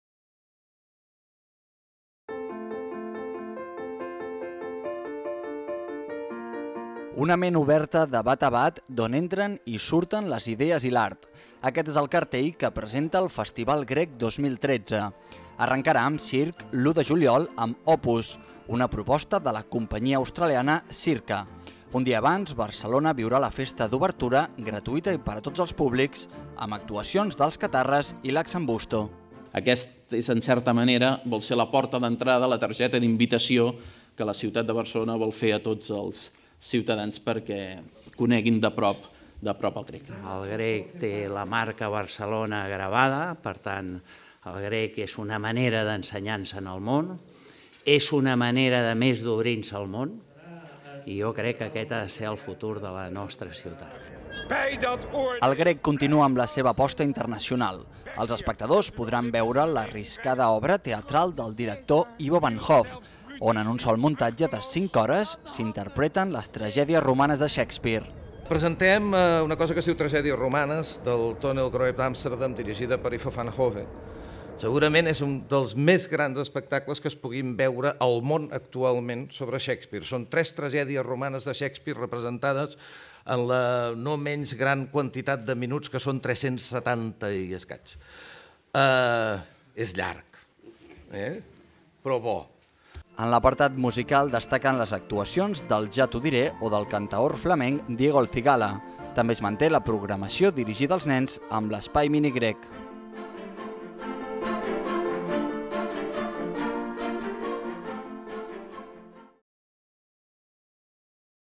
El Festival Grec 2013 es presenta amb la 'ment oberta'
De l'1 al 31 de Juliol, el festival aplegarà 79 propostes de teatre, dansa, música i circ. Hi haurà apostes arriscades com l'obra teatre de cinc hores d'Ivo van Hove.